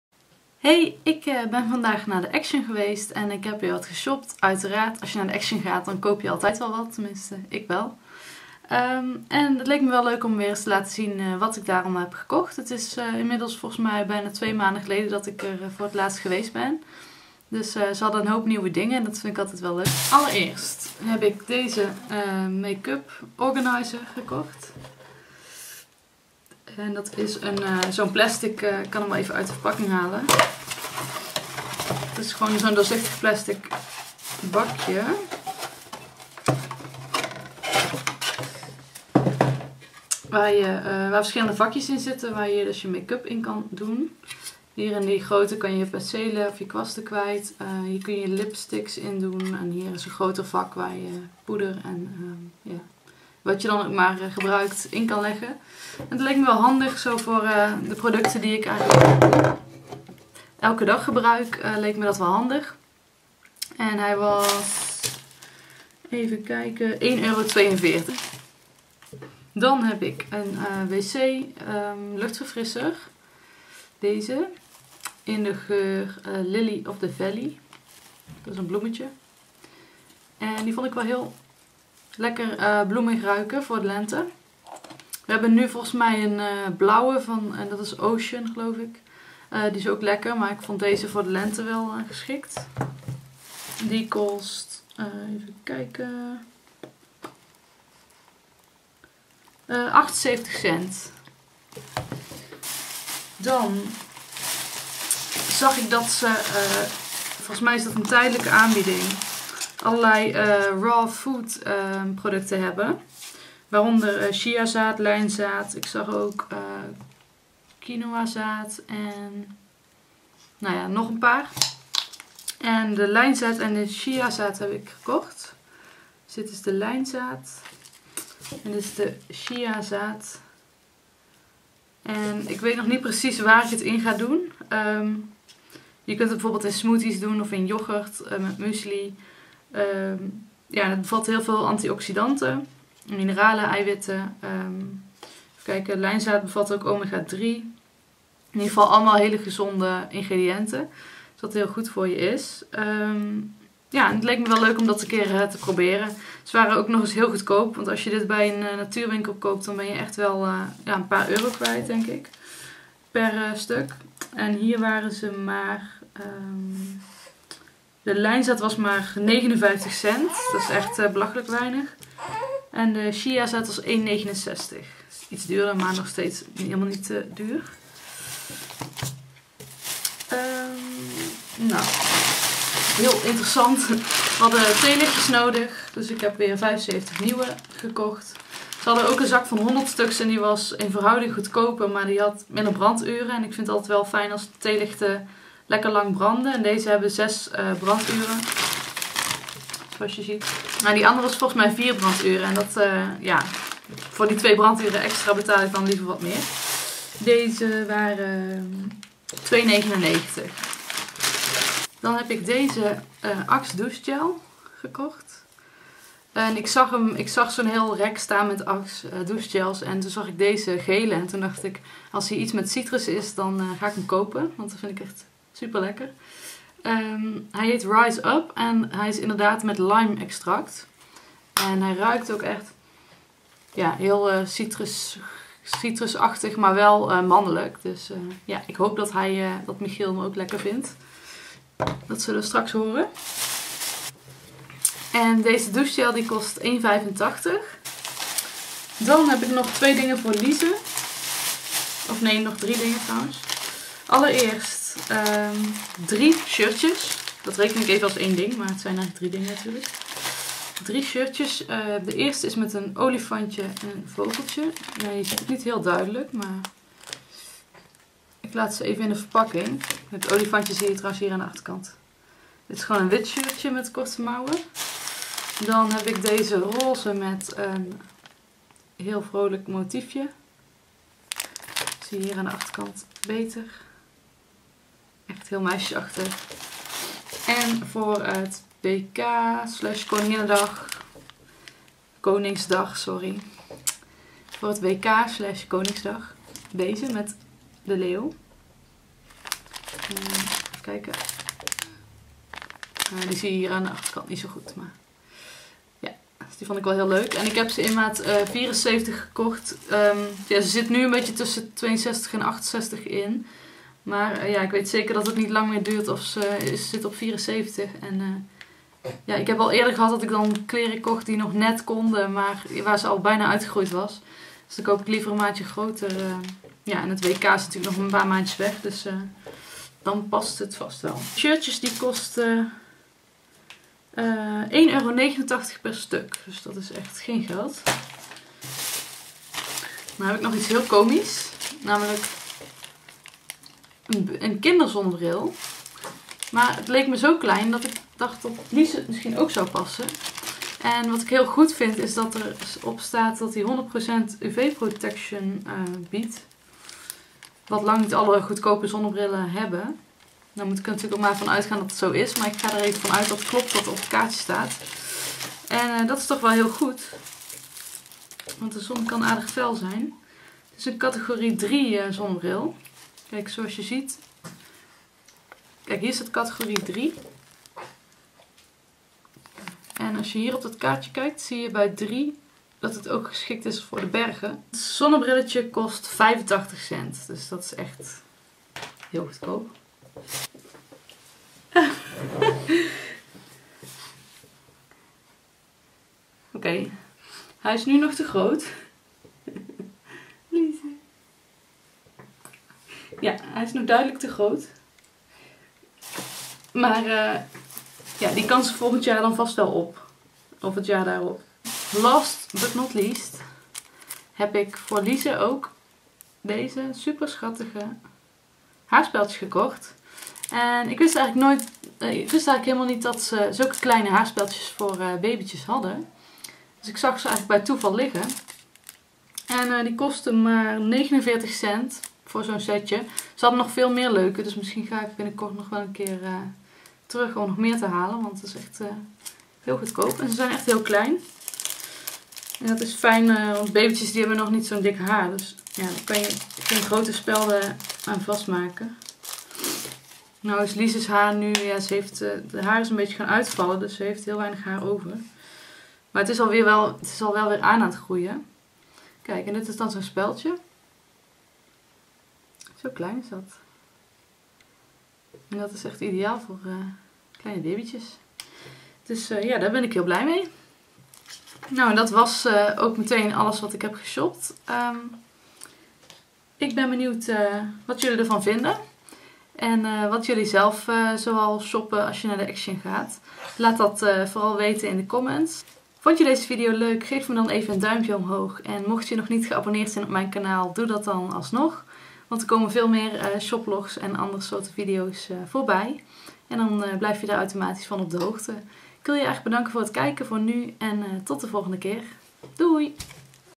ACTION SHOPLOG!
Het is weer zover... Ik ben naar de Action geweest! In deze video laat ik je mijn aankopen zien. Wat vind je ervan?